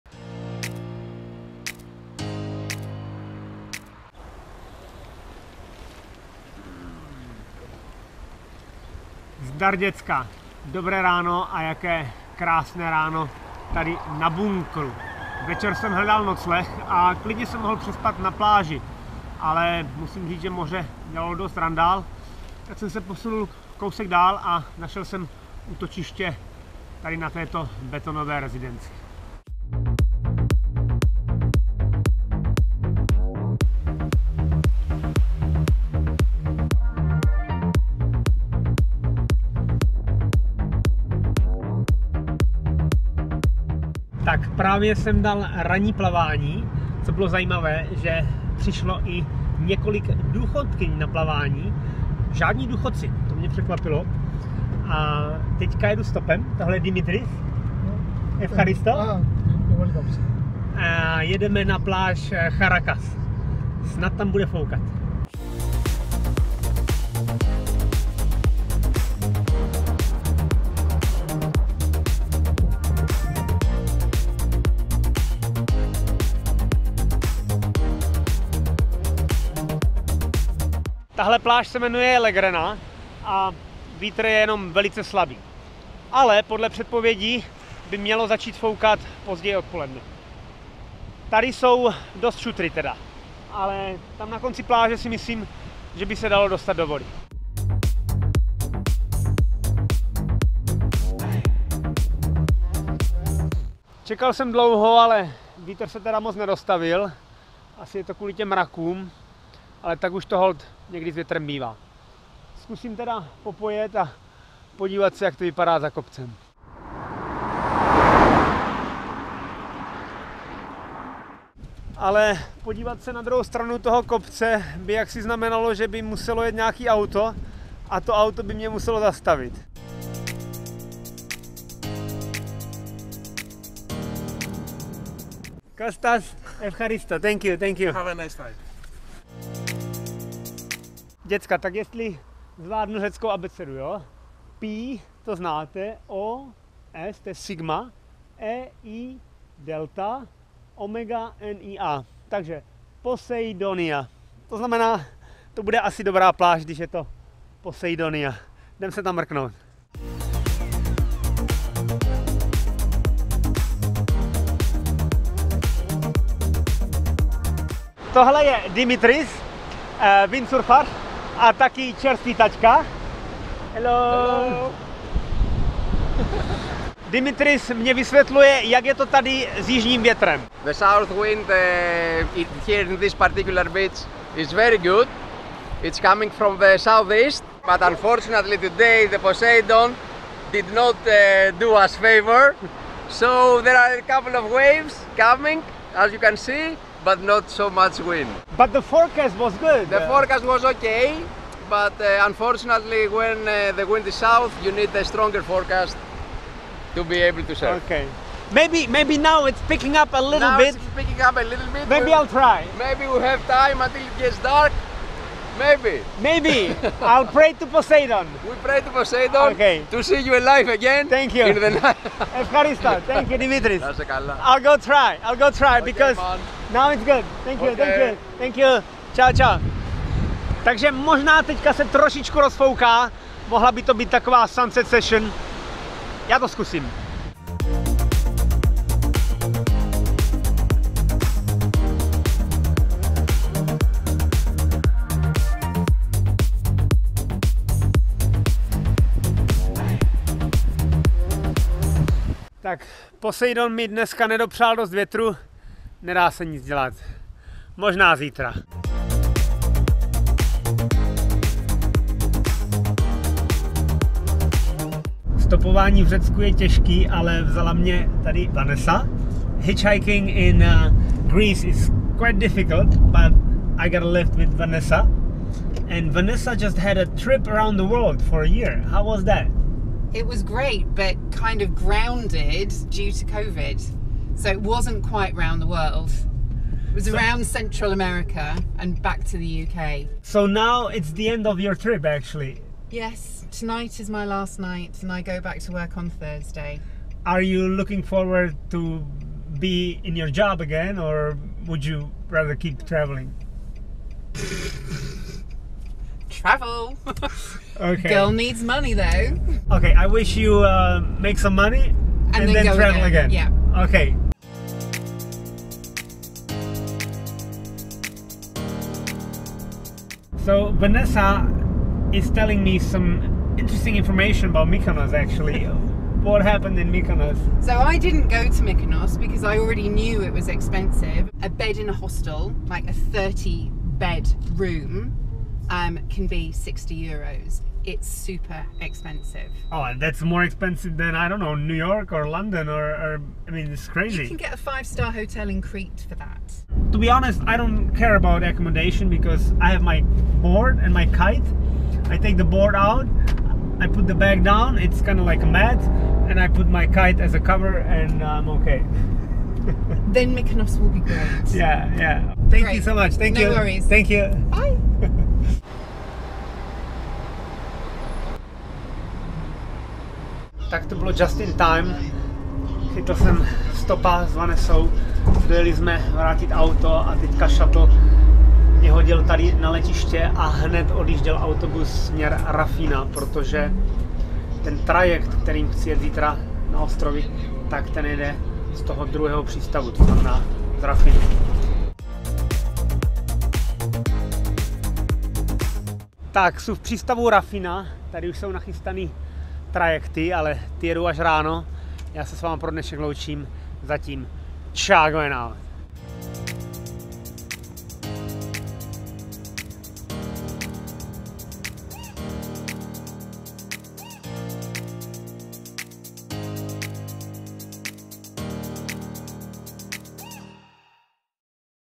Nocleh na bunkru :: VLOG 361
Možná si říkáte, že usínat přímo na pláži musí být ta největší romantika.  Ne však pokud je moře neklidné!  To se potom těžko usíná.  A proto jsem se posunul pár set metrů do vnitrozemí, kde jsem našel betonový bunkr.  Celý den pak byl ve znamení hledání větru.  Bohužel nepomohl ani Poseidon, který má nedaleko chrám.  Večer mě vzala stopem Vanessa.  Končil ji právě rok cestování, ale do práce se netěšila.  Za mlada jsem také rok cestoval, zjistil jsem však, že rok je moc dlouhá doba.  Nyní se mi líbí kombinace, kdy mohu doma něco budovat a příležitostně cestovat.  Napište do komentářů, jestli byste  chtěli rok jen cestovat.